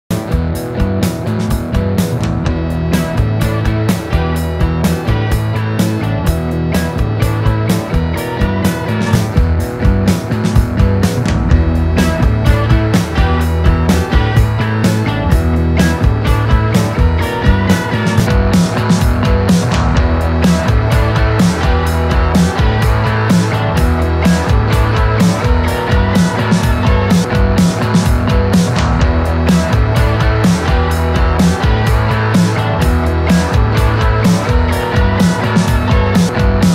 Antes y después 3 etapas
Comparación desde  1 Pistas en bruto , grabadas en Home Estudio con una mezcla estática , equilibrio de elementos a 2 Mezcla para luego exportar Stems a 3 Mezcla de Stems y Mastering